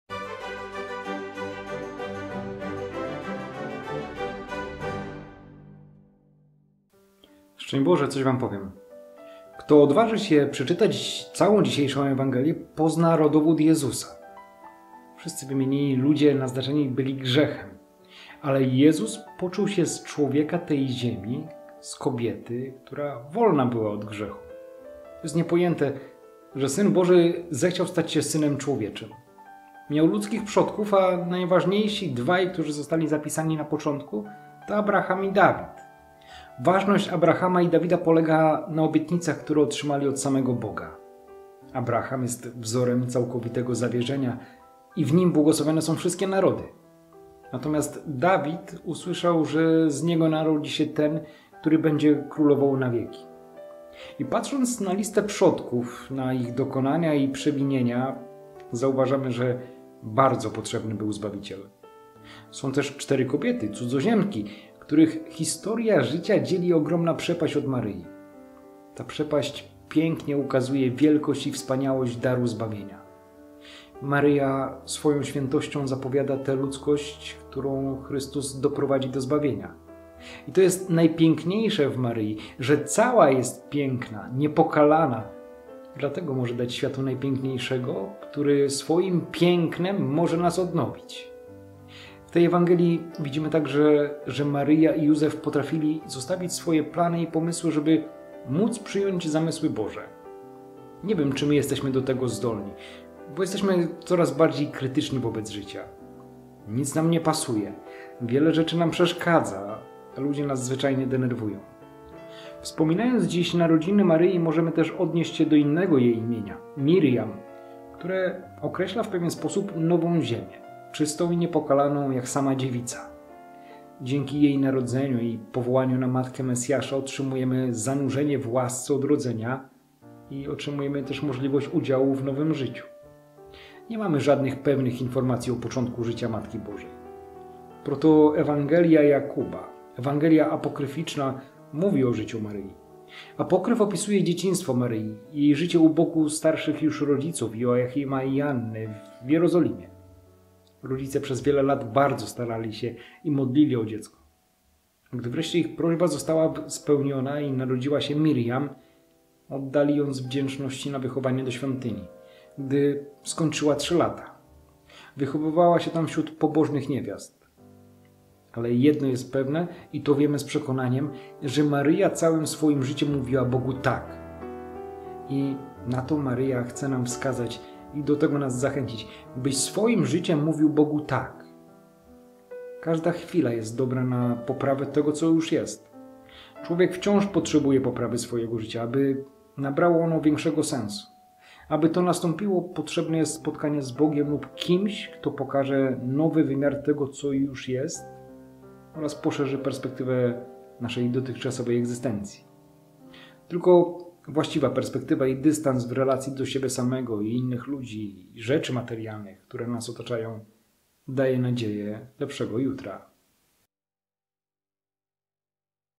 coswampowiem Mt 1, 1-16. 18-23 || 8 września 2022
Każda chwila jest dobra na poprawę tego, co już jest. Człowiek wciąż potrzebuje poprawy swojego życia, aby nabrało ono większego sensu.